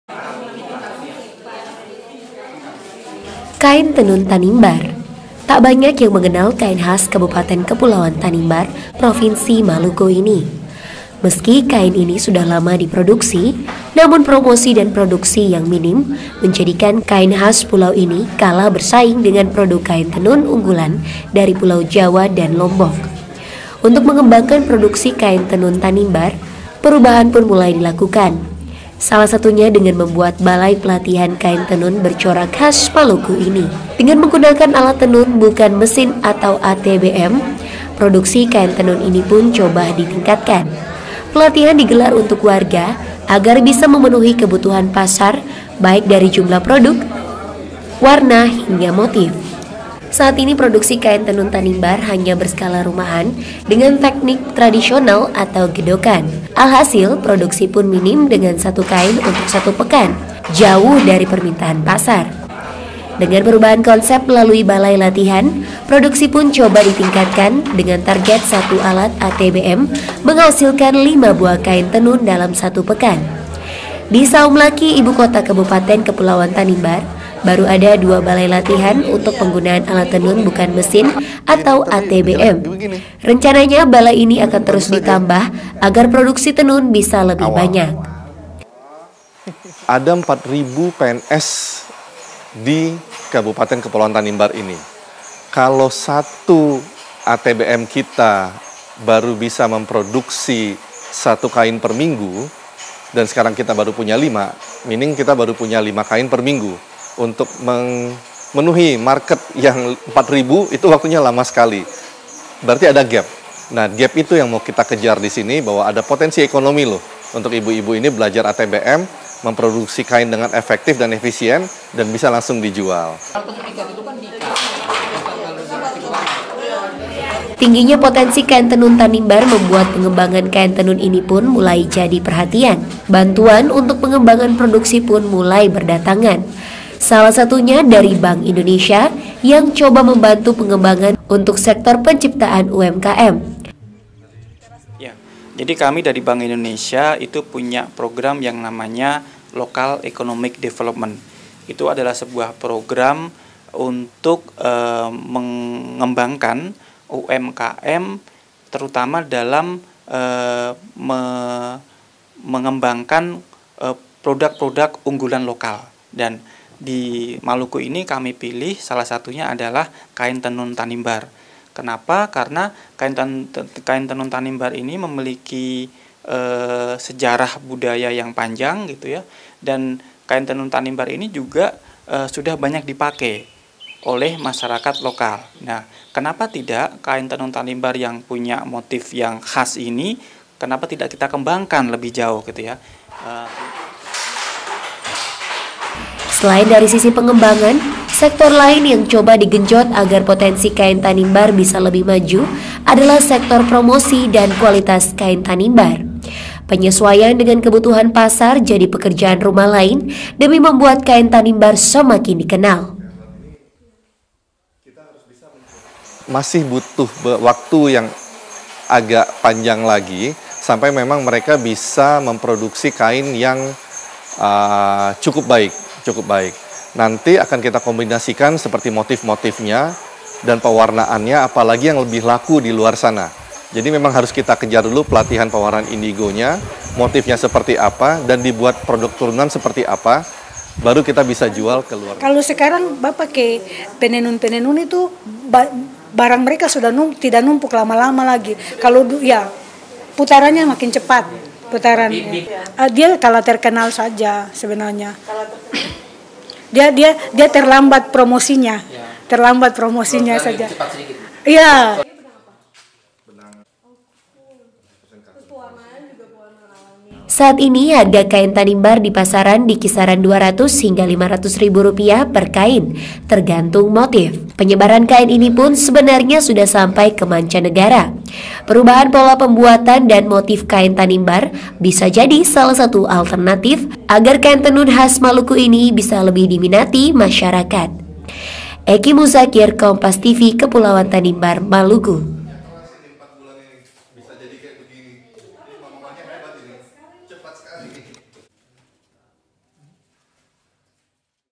PENGEMBANGAN KAIN TENUN TANIMBAR
Kepulauaan Tanimbar Di Provinsi Maluku, Menyimpang Potensi Kain Tenun Yang Indah Dan Memiliki Potensi Pasar Yang Tinggi.Namun,Pengembangan Kain Tenun Tanimbar Yang Masih Banyak Secara Tradisional,Membuat Produksi Kain Tanibar Tak Seimbang Dengan Permintaan Pasar. Pengembangan Kain Tanimbar Pun Coba Dilakukan Dengan Memberikan Bantuan Berupa Pelatihan Hingga Alat Tenun Yang Lebih Modern .